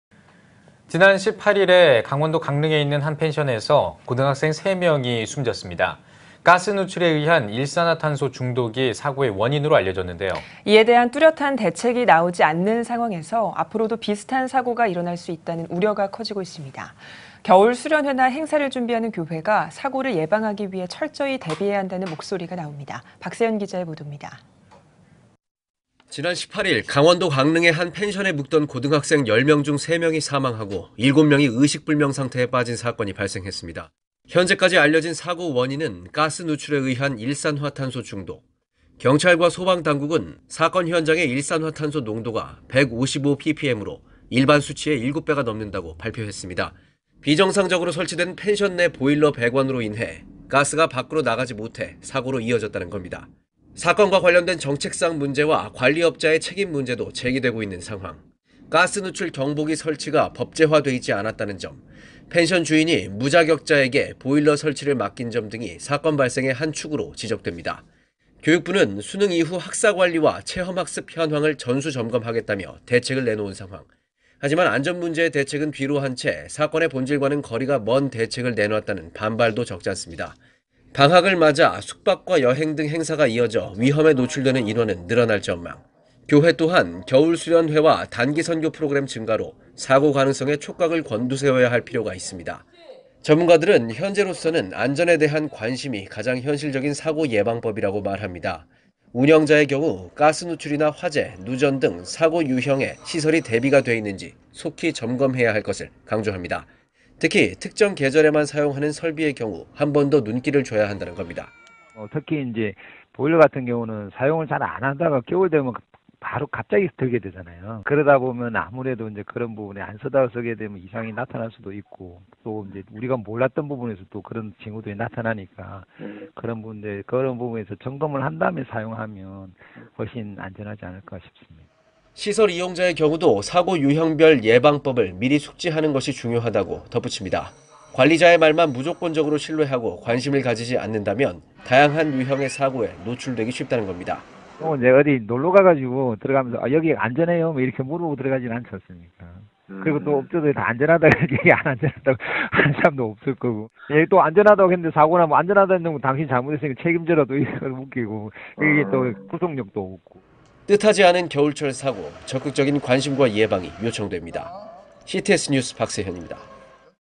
강원도 강릉 펜션 사고, 이렇다 할 대책 없어 교회도 우려 (CTS, CTS뉴스, 기독교방송, 기독뉴스, 기독교뉴스, 교계뉴스)
앵커: 지난 18일 강원도 강릉의 한 펜션에서 고등학생 10명 중 3명이 사망하는 사건이 발생했습니다. 사고의 원인은 가스 누출에 의한 ‘일산화탄소 중독’으로 알려졌는데요.

앵커: 이렇다 할 대책이 나오지 않은 채 향후 비슷한 사고 위험에 노출될 가능성이 있어 우려가 커지고 있습니다. 겨울철 수련회나 행사 준비에 한창인 교회들도 긴장을 늦춰선 안된다는 목소립니다. 박세현 기자의 보도입니다.

지난 18일, 강원도 강릉의 한 펜션에 묵던 고등학생 10명 중 3명이 사망하고 7명이 의식불명 상태에 빠진 사건이 발생했습니다.

현재까지 알려진 사고 원인은 가스 누출에 의한 일산화탄소 중독. 경찰과 소방당국은 사건 현장의 일산화탄소 농도가 155ppm로 일반수치의 7배가 넘는다고 발표했습니다. 비정상적으로 설치 된 펜션 내 보일러 배관으로 인해 가스가 밖으로 나가지 못해 사고로 이어졌다는 겁니다.

사건과 관련된 정책상 문제와 관리업자의 책임 문제도 제기되고 있는 상황. 가스누출경보기가 법제화 돼 있지 않았다는 점. 펜션 주인이 무자격자에게 보일러 설치를 맡긴 점 등이 사건 발생의 한 축으로 지적됩니다.

교육부는 수능 이후 학사관리와 체험학습 현황을 전수점검 하겠다며 대책을 내놓은 상황. 하지만 안전문제의 대책은 뒤로 한 채 사건의 본질과는 거리가 먼 대책을 내놨다는 반발도 적잖습니다.

방학을 맞아 숙박과 여행 등 행사가 이어져 위험에 노출되는 인원은 늘어날 전망. 교회 또한 겨울 수련회와 단기 선교 프로그램 증가로 사고 가능성에 촉각을 곤두세워야 할 필요가 있습니다.

전문가들은 현재로서는 ‘안전에 대한 관심’이 가장 현실적인 사고예방법이라고 말합니다.
운영자의 경우 가스누출이나 화재, 누전 등 사고 유형에 시설이 대비가 돼 있는지 속히 점검해야 할 것을 강조합니다. 특히 특정 계절에만 사용하는 설비의 경우 한번 더 눈길을 줘야 한다는 겁니다

전화 INT 한국가스안전공사 관계자

시설 이용자의 경우도 사고 유형별 예방법을 미리 숙지하는 것이 중요하다고 덧붙입니다. 관리자의 말만 무조건적으로 신뢰하고 관심을 가지지 않는다면 다양한 유형의 사고에 노출 되기 쉽다는 겁니다.

뜻하지 않은 겨울철 사고. 적극적인 관심과 예방이 요청됩니다. cts뉴스 박세현입니다.